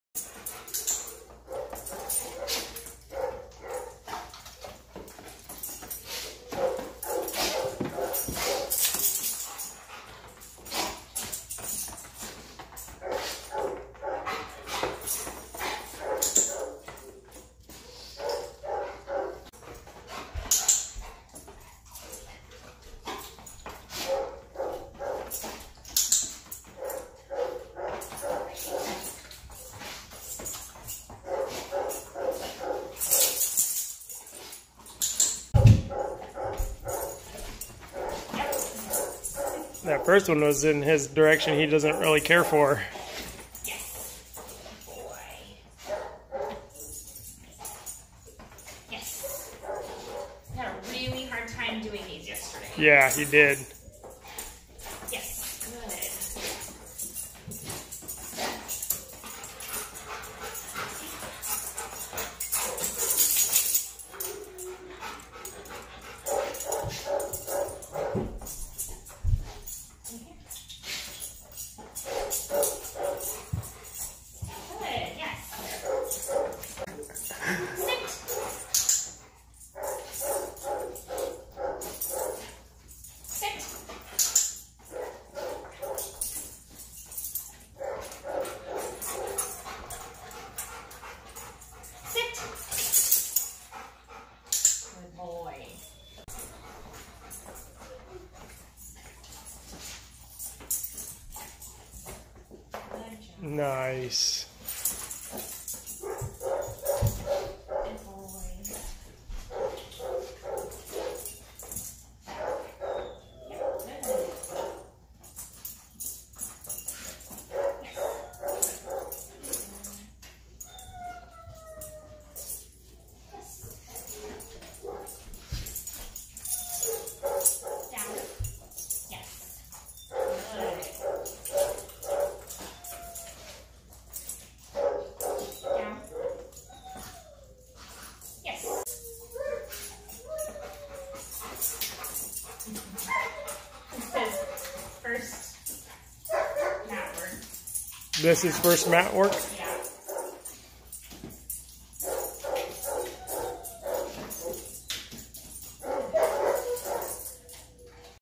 Board & train, Magnus.
Magnus is a 2 year old lab here for our board and train program. He came in only knowing "sit" but not consistently obeying that command. He's a very energetic guy with some really bad habits. He's made great progress so far but there's definitely still work to do!